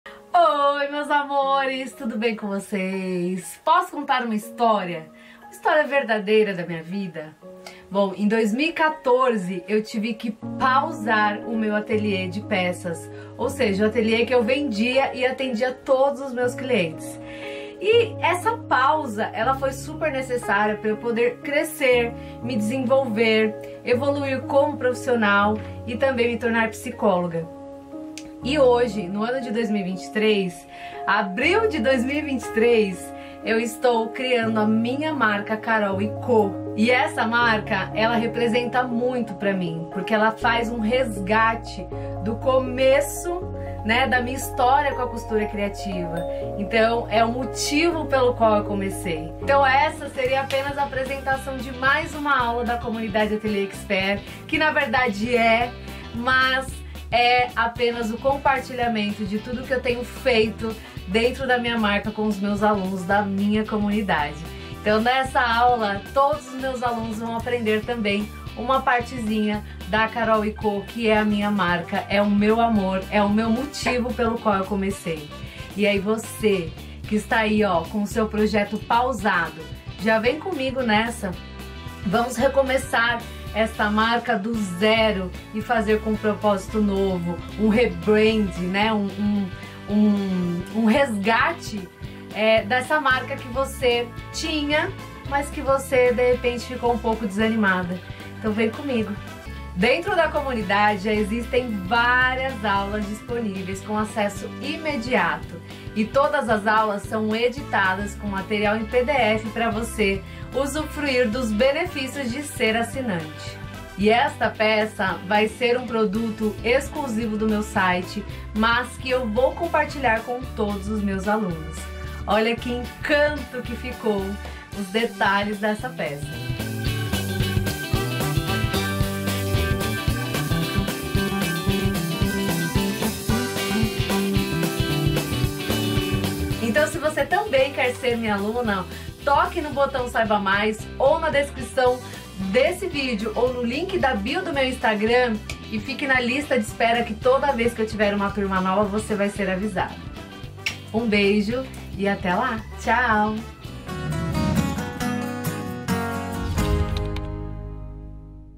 AULA 210 MALA MULTIUSO - COMUNIDADE ATELIÊ EXPERT - CAROL VIANA
Nessa semana no Ateliê Expert você vai aprender a fazer esta esta peça.
Essa é uma peça que faz parte de um grande sonho e que vou compartilhar com meus alunos.   É uma mala super moderna.
Essa é uma aula exclusiva para assinantes.